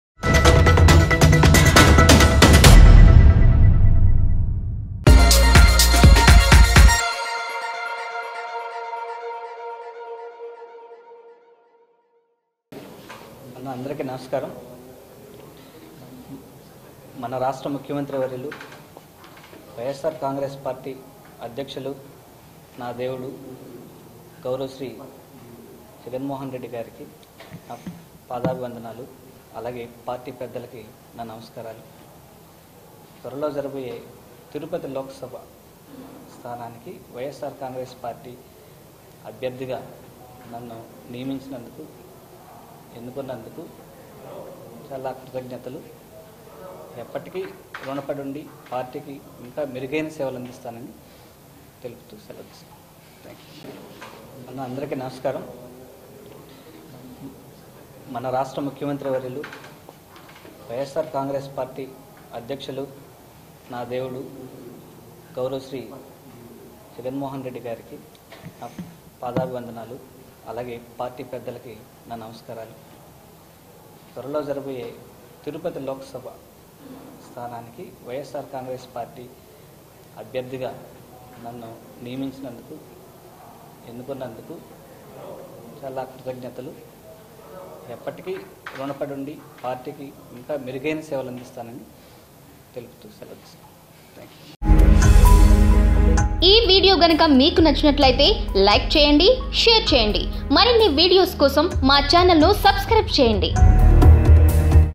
Tirupati By Elections | Ys Jagan | YSRCP Candidate Gurumoorthy |మొదటిసారి మీడియా ముందుకు గురుమూర్తి
మొదటిసారి మీడియా ముందుకు గురుమూర్తి